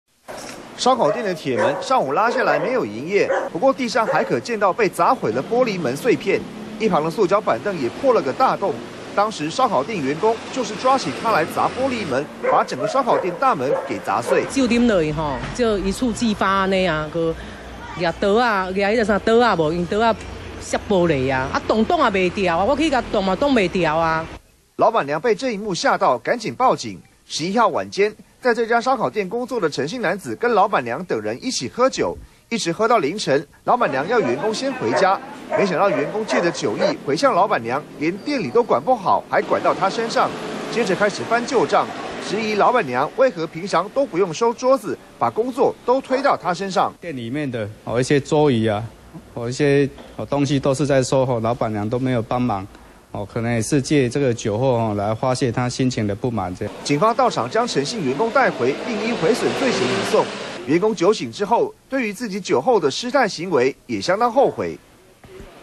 嗆老闆娘不收桌 員工砸店耍流氓
中視新聞》台中市燒烤店一名員工，喝完酒後和老闆娘起了口角，抱怨老闆娘把工作都推給他，一氣之下竟然拿起板凳砸破燒烤店的玻璃大門，而老闆娘被嚇得趕緊報警處理。